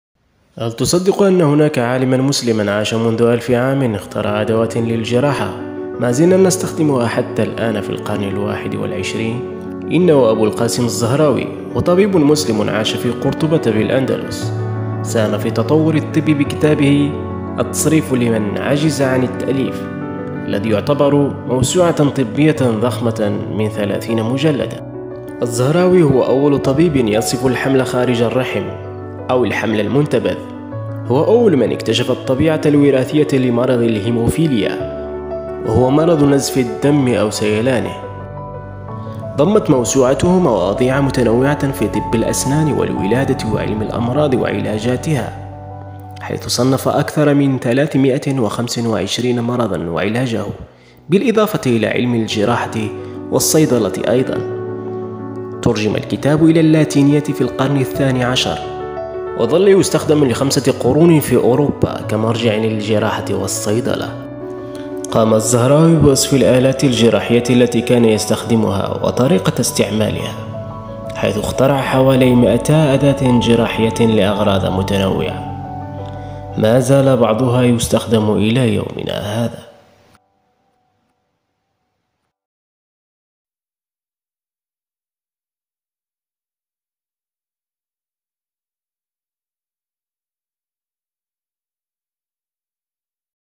أبو القاسم الزهراوي | المؤسس الحقيقي لعلم الجراحة .
أبو القاسم الزهراوي عالم عربي مسلم ولد بالاندلس في قرطبة ..مؤسس علم الجراحة وأول من اخترع ادواتها ..طبيب ومعالج عظيم ساهم في تطور الطب في عصره ومازالت اختراعاته تستخدم إلى يومنا هذا